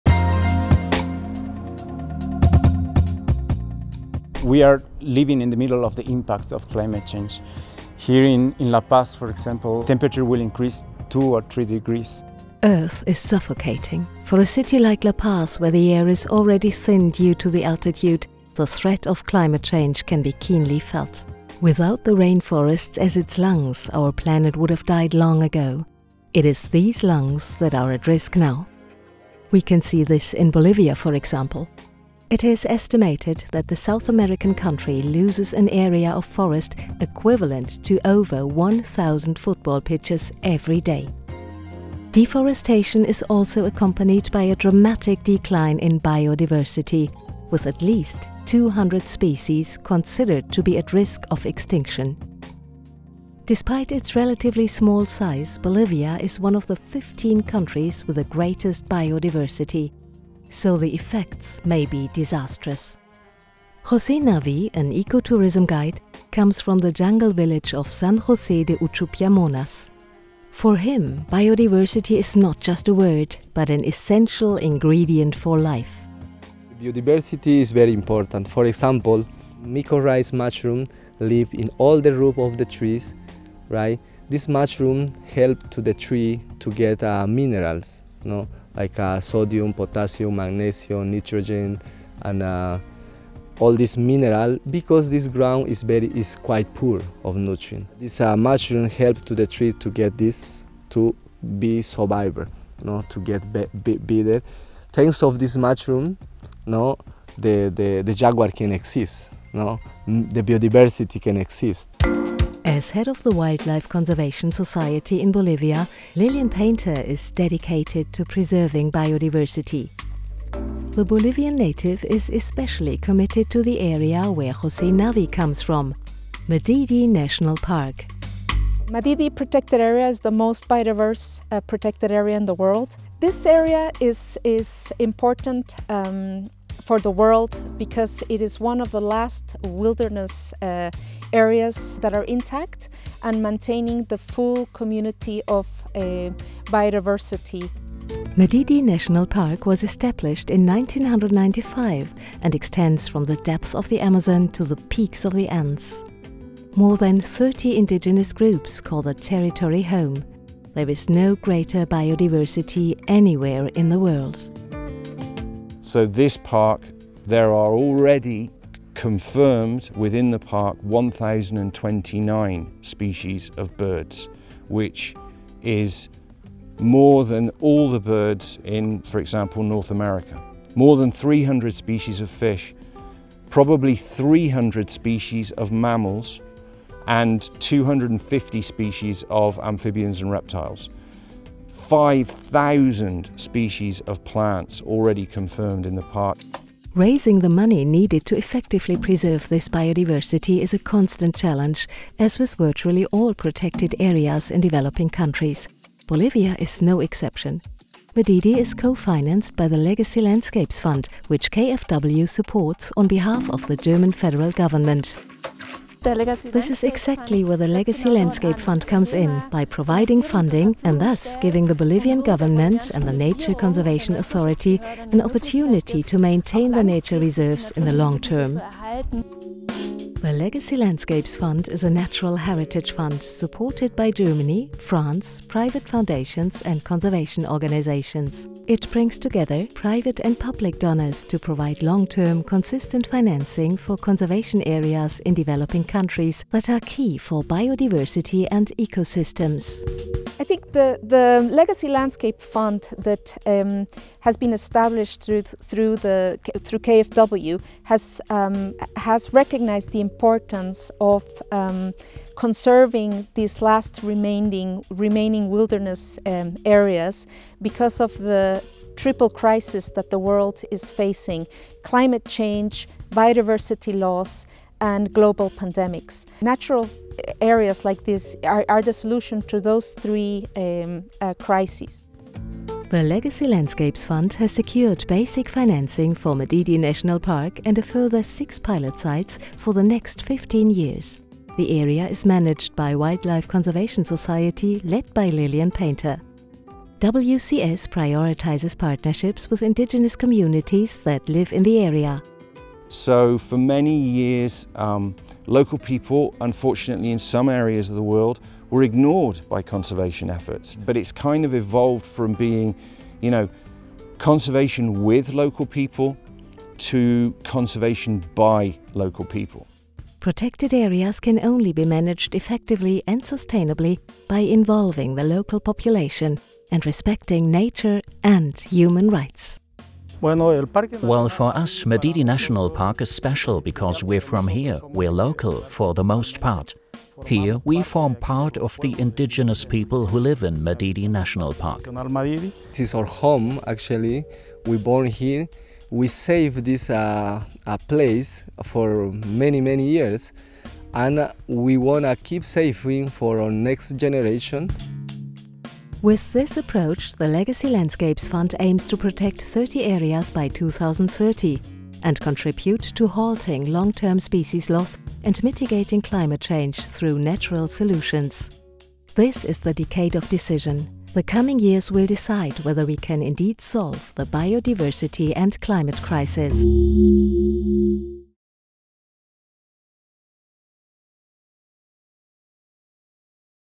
Madidi National Park – KFW (English)
A video exploring Madidi National Park, one of LLF's legacy landscapes.